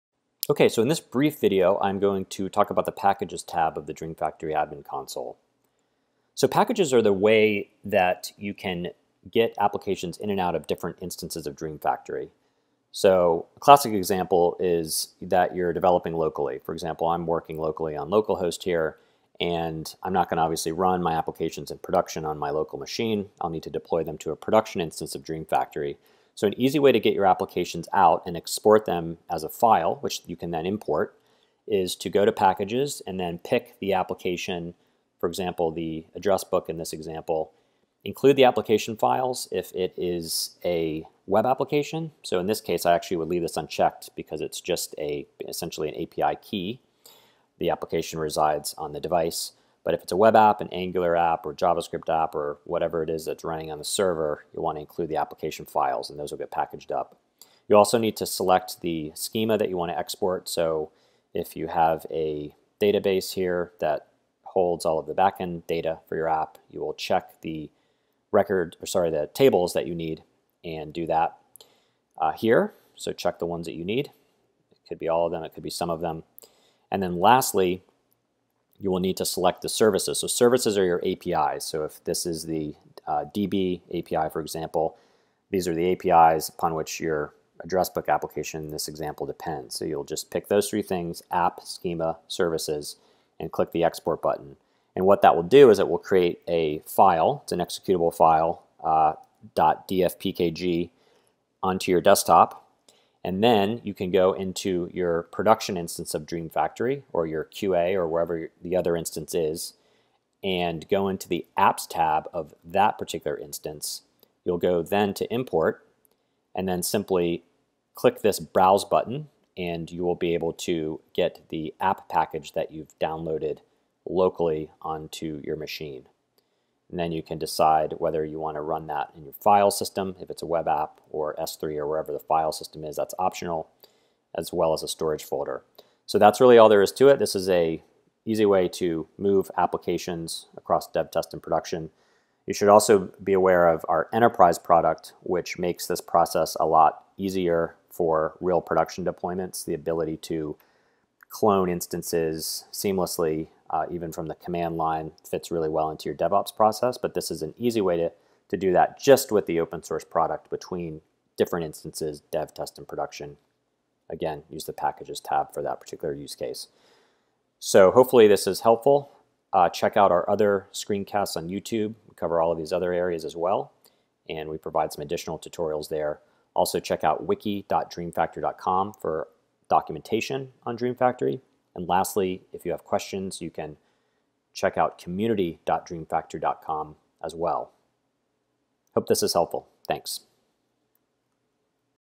DreamFactory Admin Console: How to Use the Packages Tab
This tutorial provides a short overview of the Packages tab in the DreamFactory Admin Console.